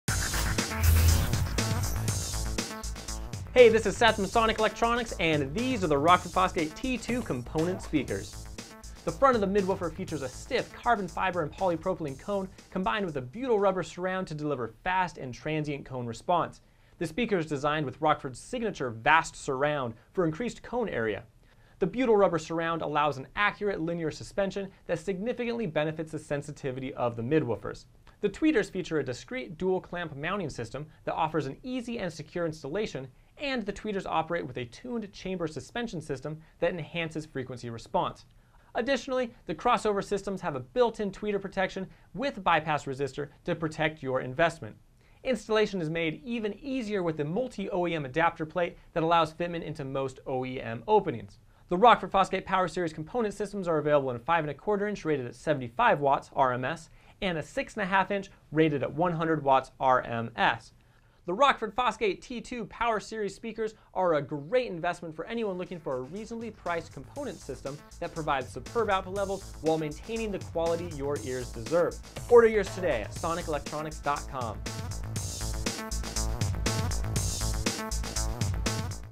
Rockford Fosgate T2 Component Speakers | Power Series Loudspeakers
See the Rockford Fosgate T2 Aluminum Component Speakers up close! In this video we give a quick review of the Power series T2 component systems from Rockford Fosgate. These higher-end speakers feature audiophile grade components, carbon fiber & polypropylene cone, butyl rubber surround, aluminum done neo-tweeter, FlexFit baskets, Discreet Dual Clamp (DDC) tweeter mounting, Vertical Attach Surround Technique (VAST) surround, PTC protection, tweeter attenuation, CEA-2031 rating, and more!